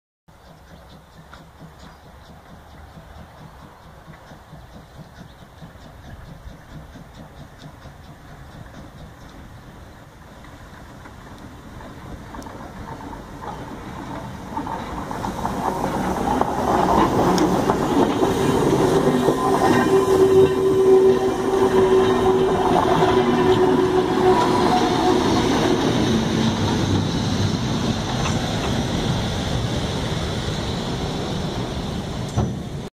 Ankunft Dampfbahn Fränkische Schweiz Lokomotive 8 (64 491)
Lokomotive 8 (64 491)
Bauart: 1´C1´h2t
Baujahr: 1940
Leistung: 700 kW / 950 PS
Höchstgeschwindigkeit: 90 km/h
Länge über Puffer: 12.500 mm
Achsstand: 9.000 mm
Treibraddurchmesser: 1.500 mm
Dienstgewicht: 75 t
Quelle:
Dampfbahn Fränkische Schweiz e. V.